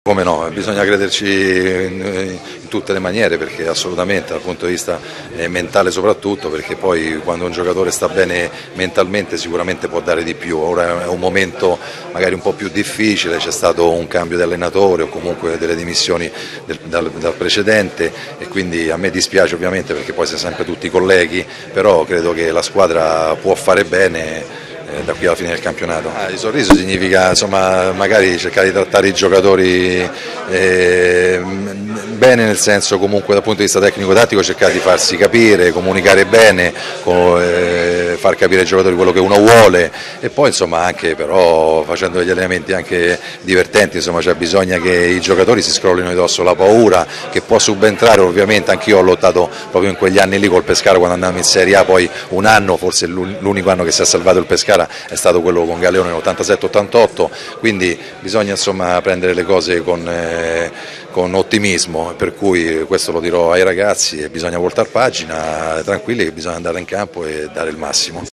VIDEO Pescara, Bergodi:|'Abbiamo i mezzi per salvarci'
"Cercherò di dare il massimo. Io sono uno che non molla mai e sono determinato in tutti i sensi. Conosco questa squadra e credo che abbia i mezzi per poter ottenere la salvezza". Queste le prime parole di Cristiano Bergodi nella conferenza stampa di presentazione nelle vesti di nuovo allenatore del Pescara. &#39;Sono emozionato - ha aggiunto Bergodi - per essere tornato in una città che mi ha voluto bene. Voglio ringraziare il presidente Sebastiani e il ds Delli Carri per avermi scelto come allenatore. Per me è motivo di soddisfazione e orgoglio essere oggi l&#39;allenatore del Pescara. A questa città mi lega un sincero affetto. Sono stato nove anni a Pescara, dall&#39;80 all&#39;89, scrivendo con questa società nel 1986-87 una pagina storica".